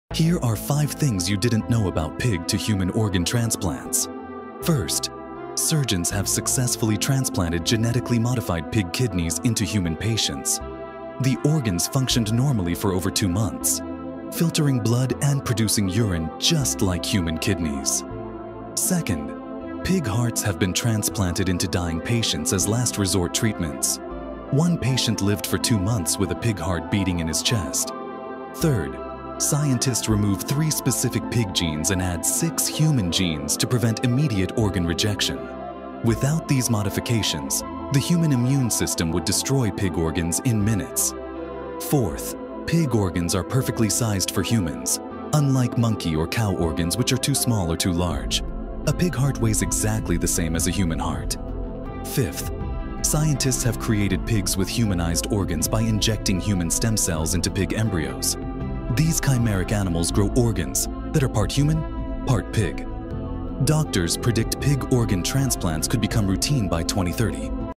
5 Pig Organ Transplant Facts That Will BLOW Your Mind
Pig organs in humans by 2030? Wild stuff